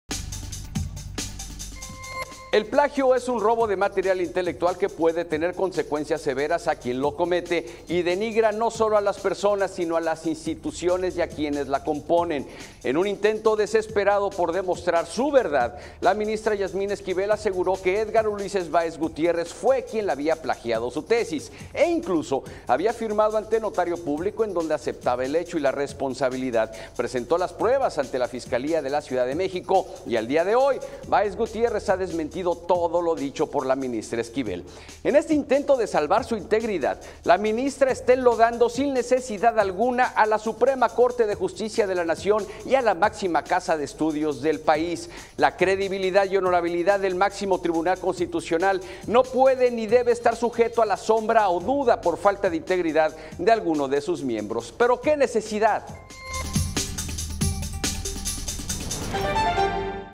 ¿Dónde está la honorabilidad?
La opinión del periodista Eduardo Manzanares en 100 palabras